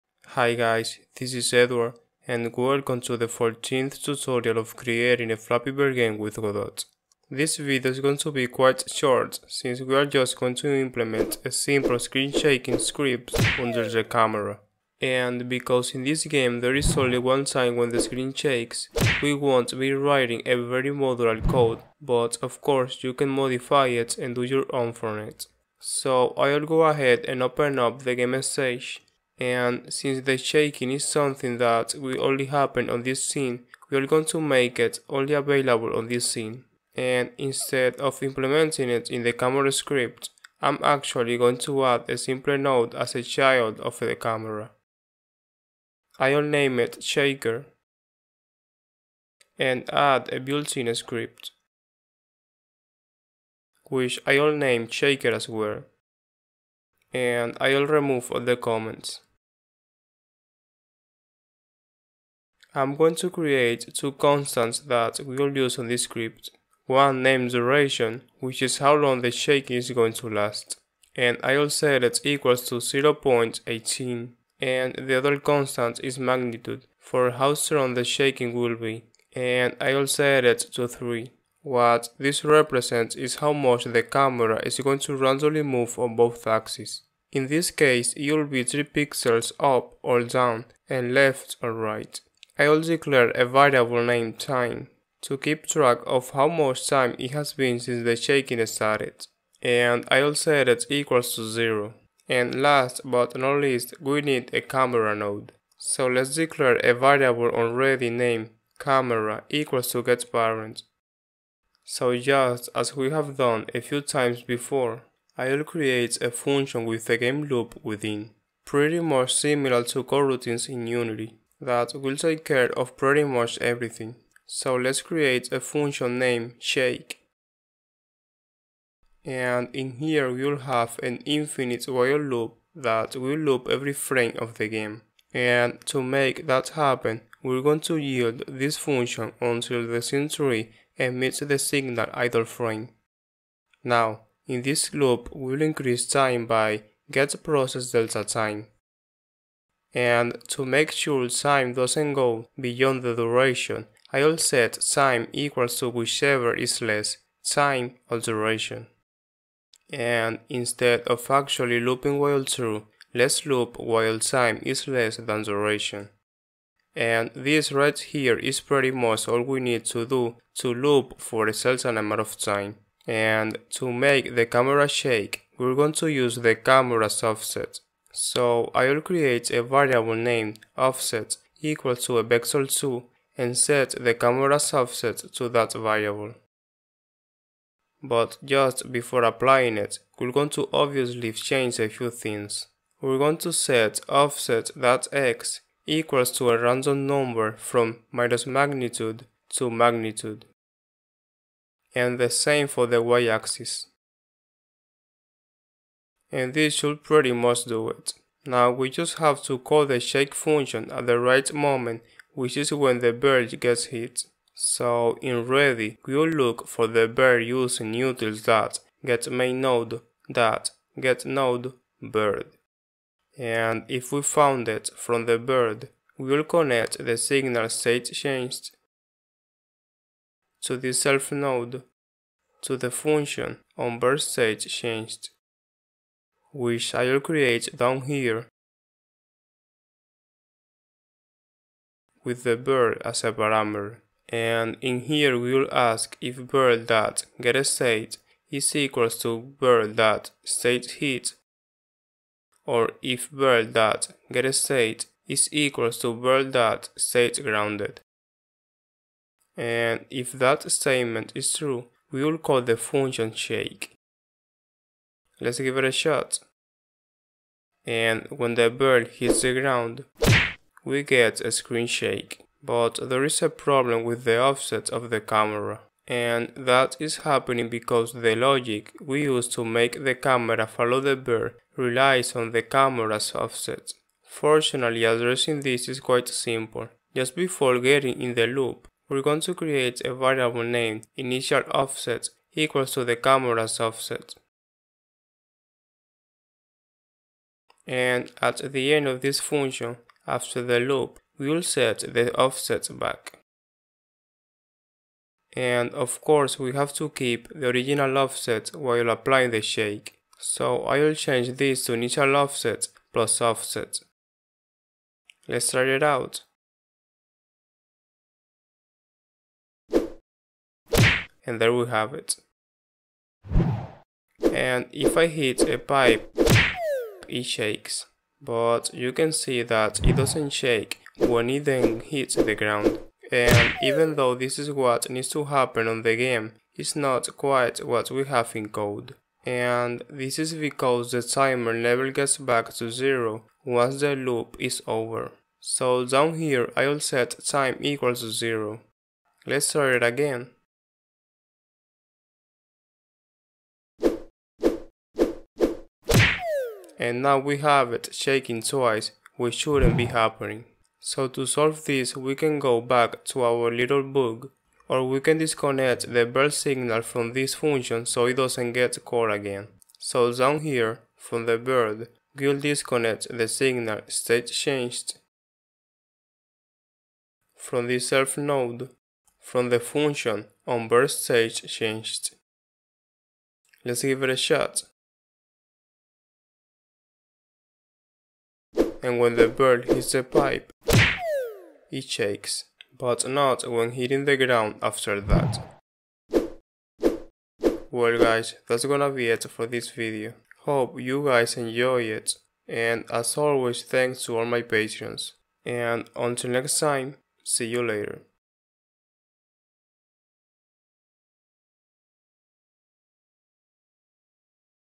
Godot Engine Tutorial - Flappy Bird - 14 Screen Shaking
In this video we make the camera shake when the bird gets hit so the game feels more alive.